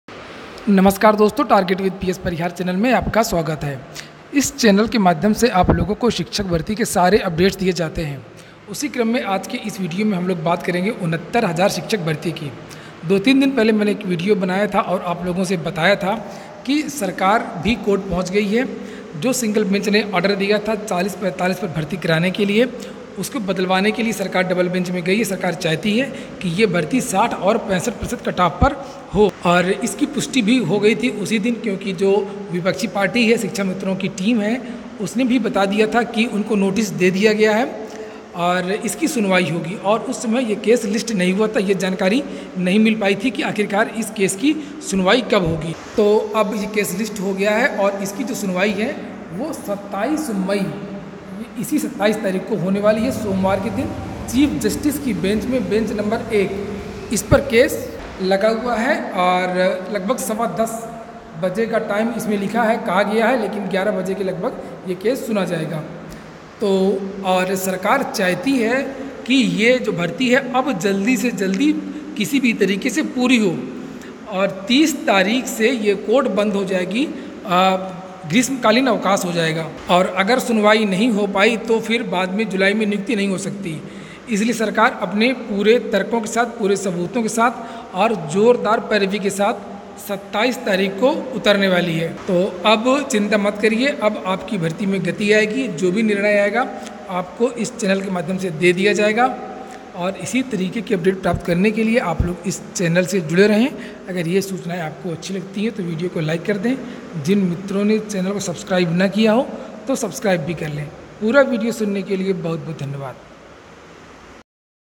69000 शिक्षक भर्ती: सरकार की अपील पर इस दिन होगी सुनवाई
69000शिक्षक भर्ती बड़ी सूचना